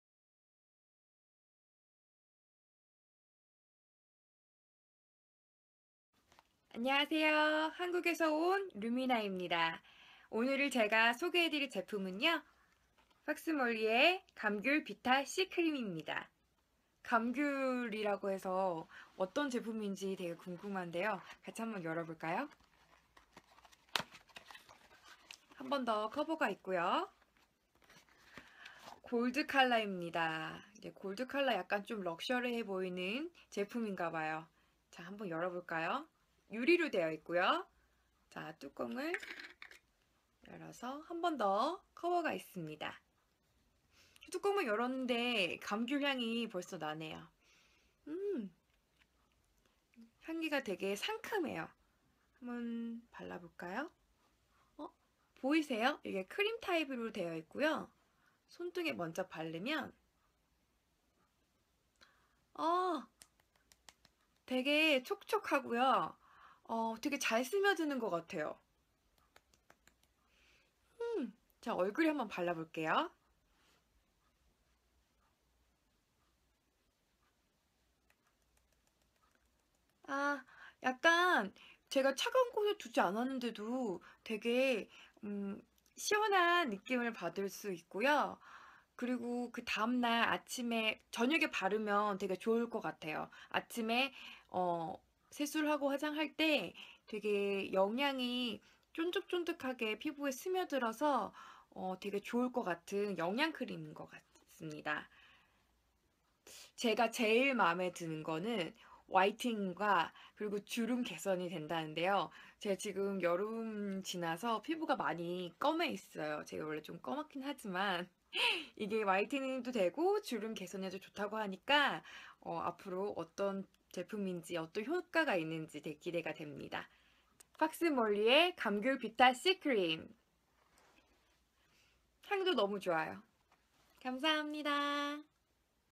PAX MOLY Vita C cream review by Korean actress Yoo Minha
BIG HIT! PAX MOLY Vita-C cream from KOREA for smooth skin! Delivery free everywhere in Nepal!

★Moisturizing / Nourishing / Soothing / Skin glow / Tone up the skin!
 ★Only this after face wash
 ★Wholesale available from 5pcs

Type: Tangerine Vita-C (Honey is available too)